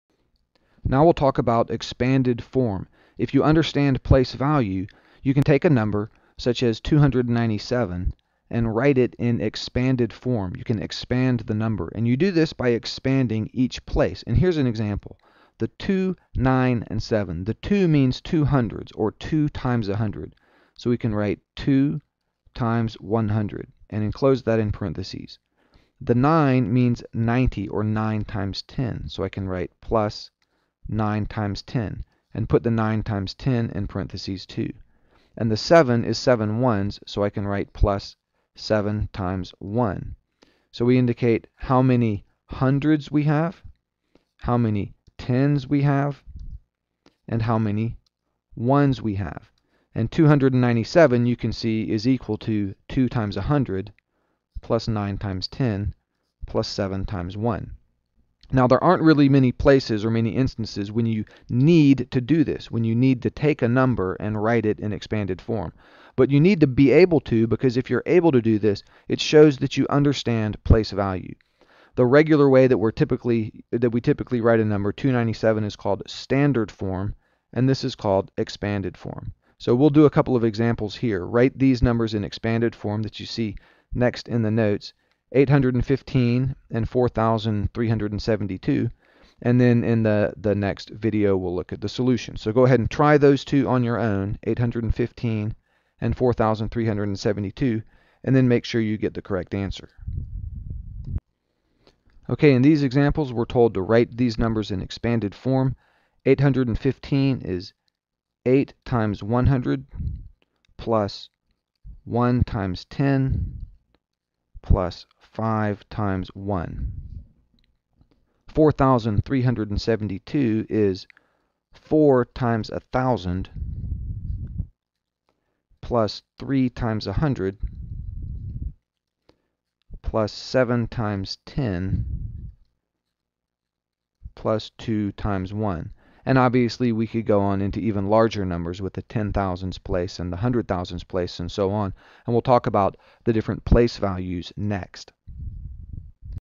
Prealgebra 1.1c - Expanded Form
Writing numbers in expanded form.  From the Prealgebra course by Derek Owens.